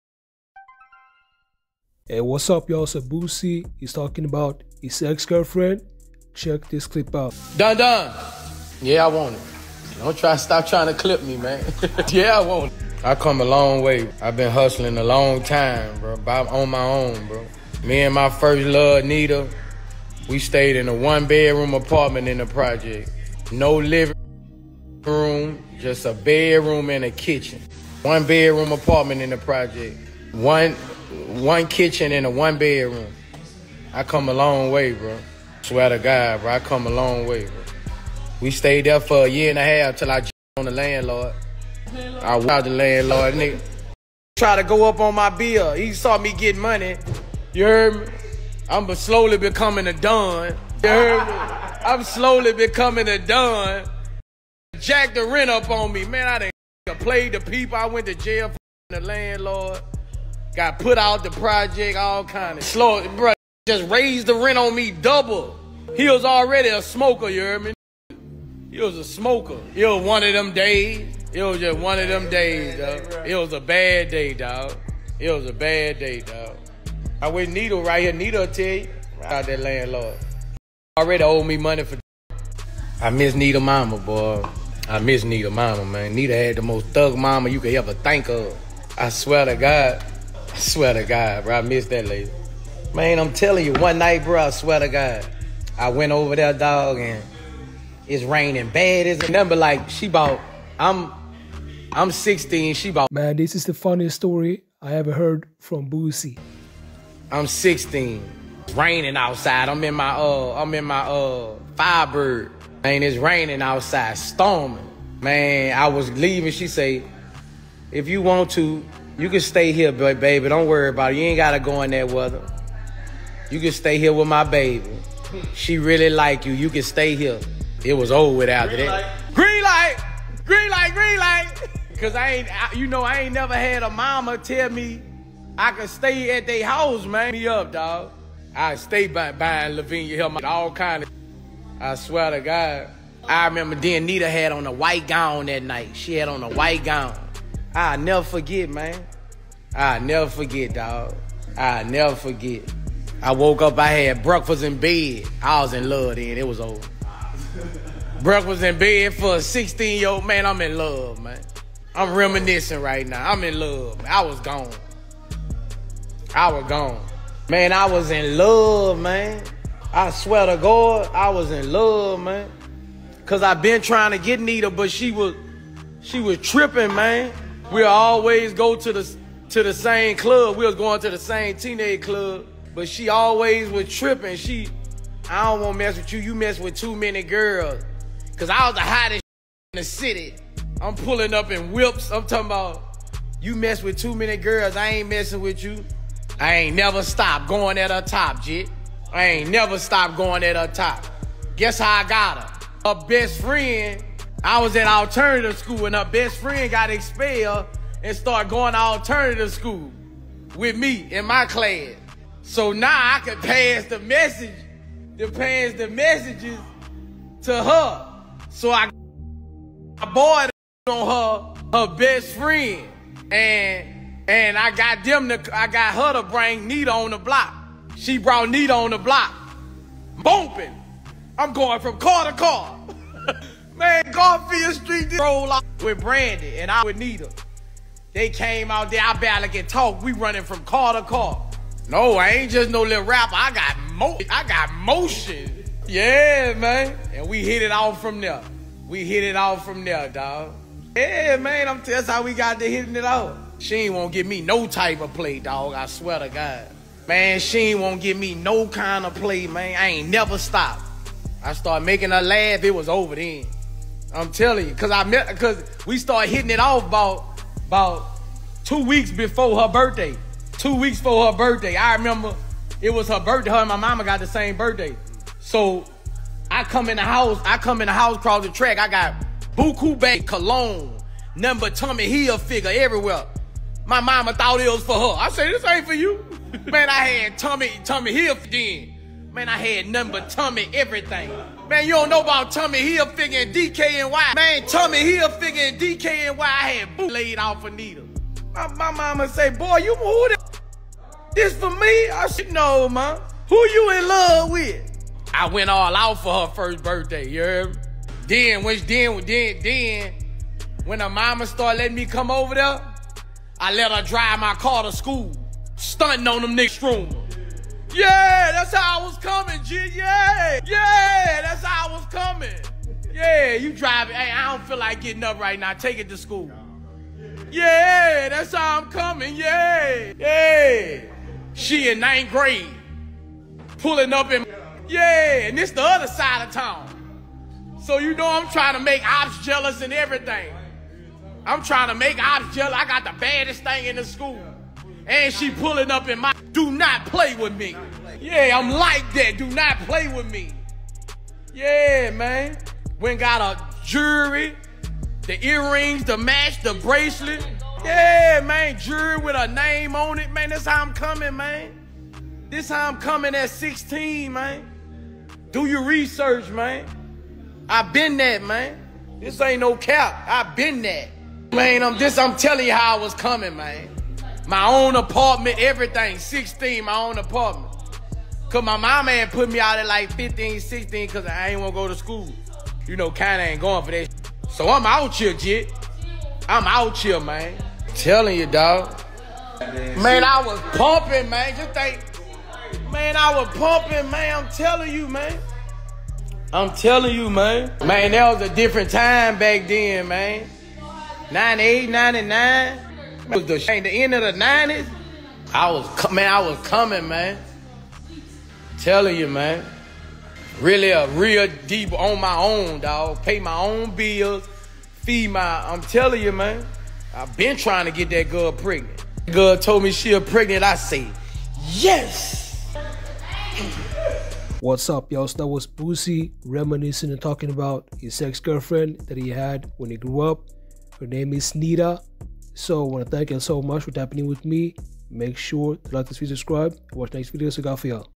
Boosie TALKS BOUT HIS FIRST BABY MAMA NITA ''WHEN SHE GOT PREGNANT I SCREAMED'
Finesse2tymes STERN WARNING To Nba Youngboy GET READY TO GET CLAPPED